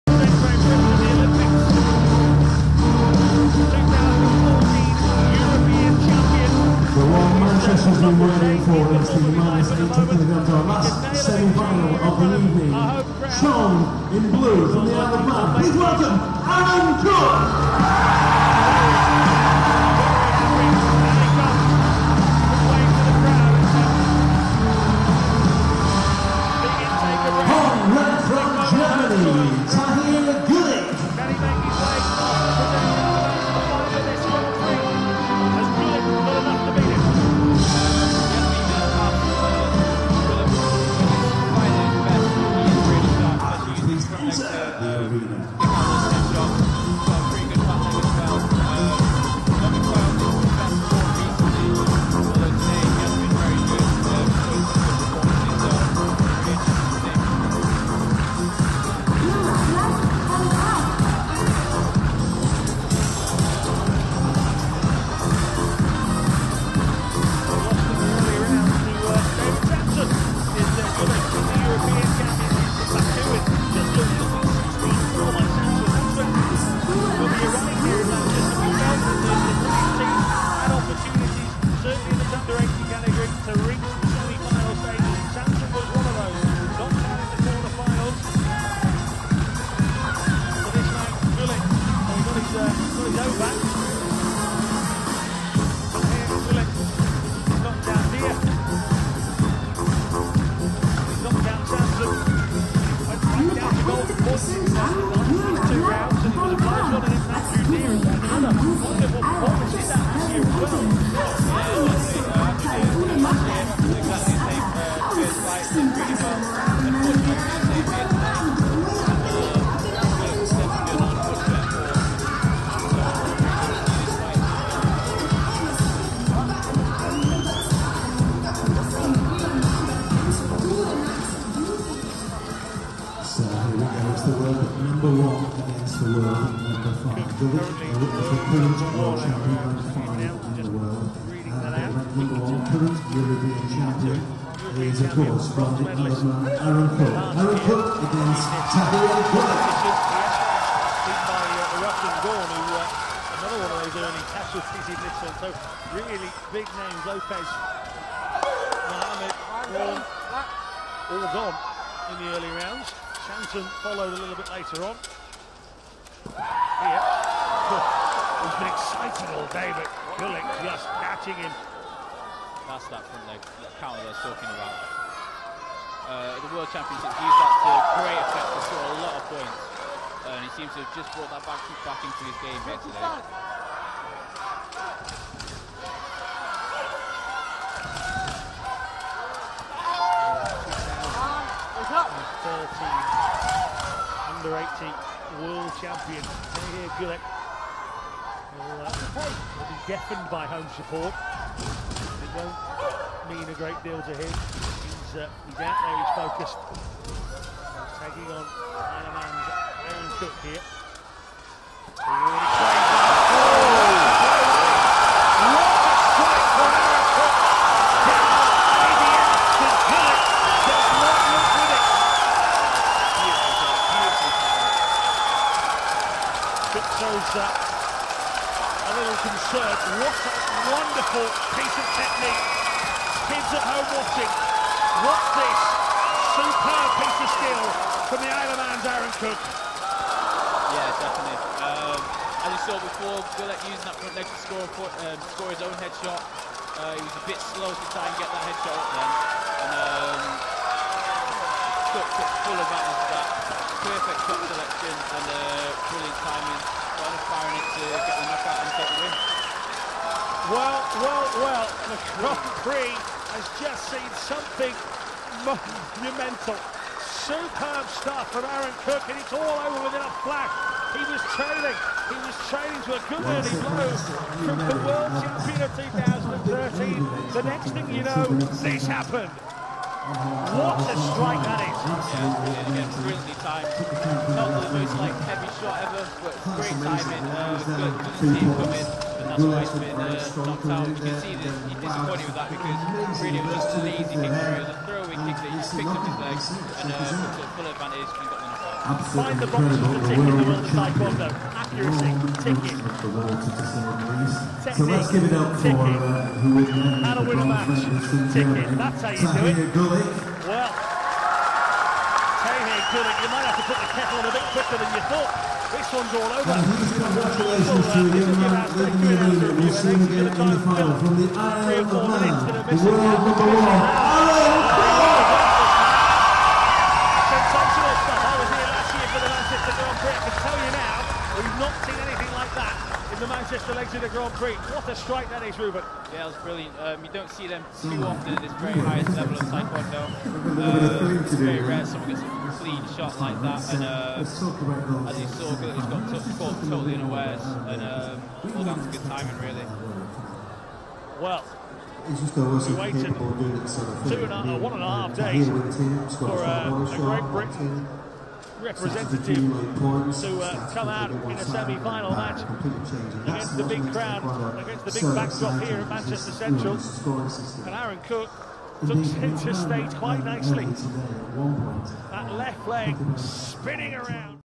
[The best game] Male -80Kg Semi Final | COOK, AARON(IMN) v GUELEC, TAHIR(GER)
Male -80Kg Semi Final | COOK, AARON(IMN) v GUELEC, TAHIR(GER)
WTF World Taekwondo GP 2014 Series 3 - Manchester